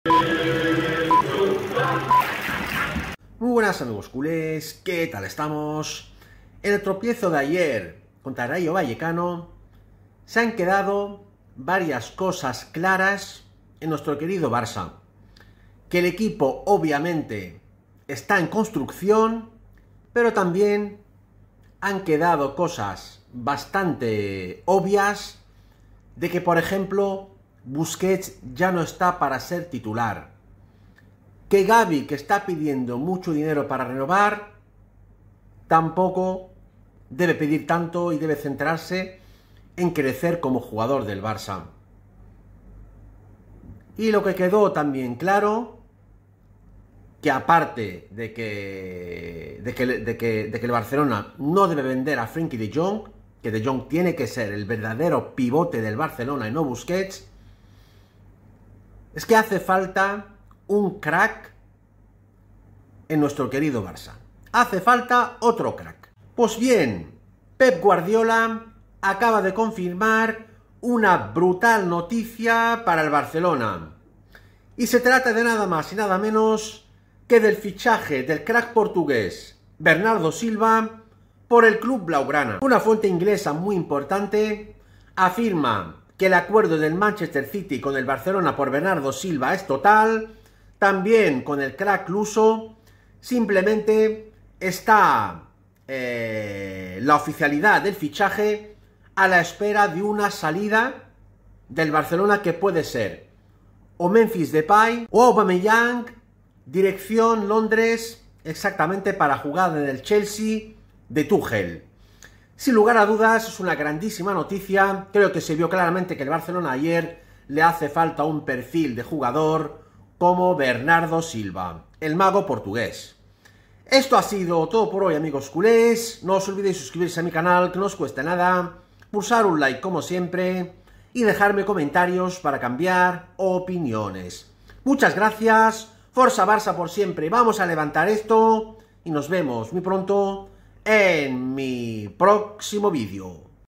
BRUTAL NOTICIA ¡¡PEP GUARDIOLA ACABA DE ANUNCIAR EL GRAN BOMBAZO DEL BARCELONA!!
Guardiola confirma una brutal noticia para el Barcelona